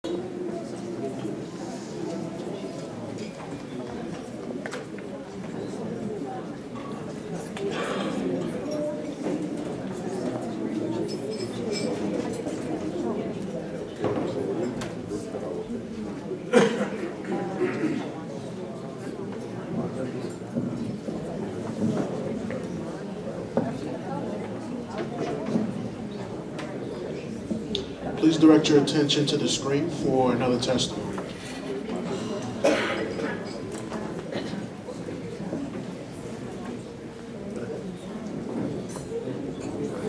WAFAA Non-Profit Fundraiser Event 2016 (Part 3)
Thank you London for the video of the WAFAA Non-Profit
Fundraiser Event on June 8, 2016.

With Eric Toledo-Drislane and Rooselte as MCs.

Break Meeting b4 Intro to Testiomony